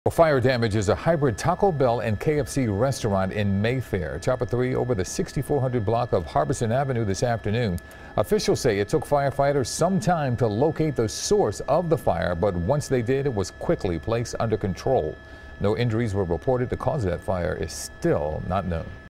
Fire At Mayfair KFC/Taco Bell
The cause of the blaze is still unknown.